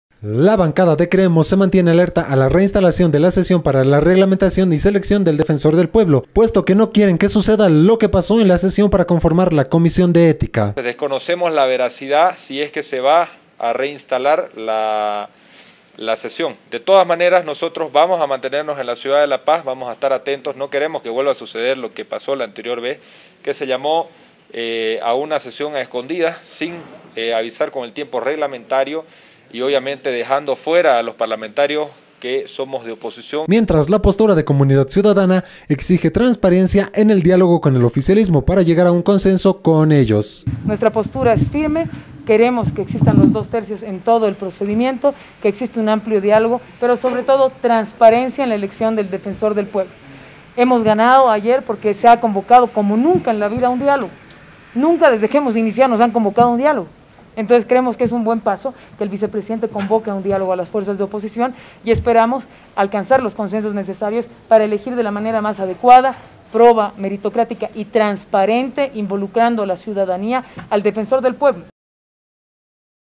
OPOSICIÓN SE MANTIENE EN VIGILIA ANTE EL CUARTO INTERMEDIO
Oposición se mantiene en vigilia ante el cuarto intermedio de la sesión del DefensorDelPueblo.